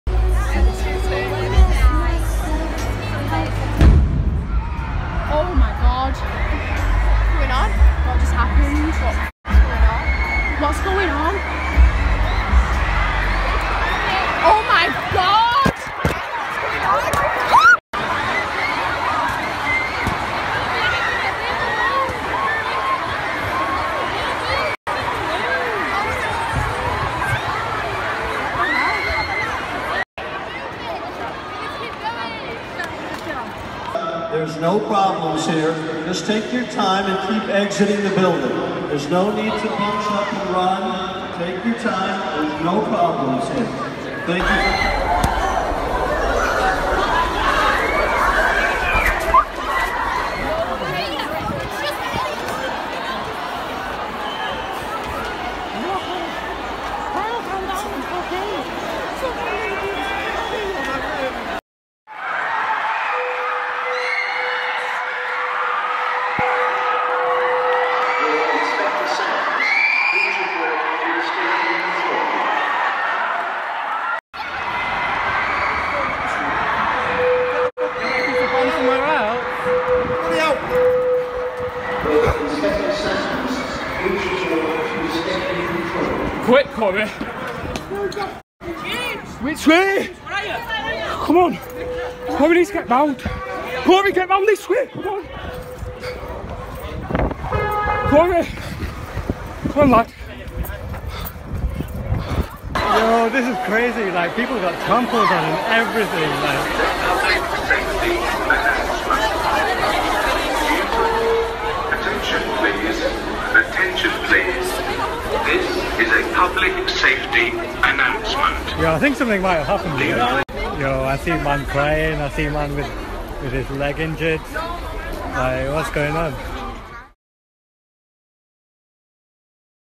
Manchester terror attack - through the camera lens
Key video from the night a suicide bomber targeted young concertgoers at an Ariana Grande gig in the Manchester Arena.